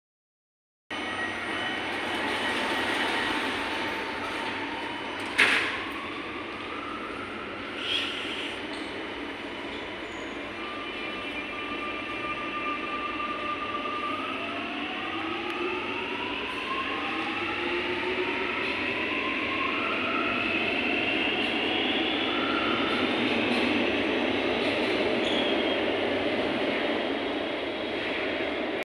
Refurbished C751A set 7009/10 departing Outram Park 💜 🚇
Outram Park MRT station is one of Singapore's Triple line stations in the MRT system 🚇 💜 💚 🤎

This video features trainset 7009/10 heading Citybound or Southwest bound towards Harbourfront 6️⃣

P. S. This refurbished train has its unrefurbished old seats being up cycles at the HDB void decks at Woodlands and Yishun ITH.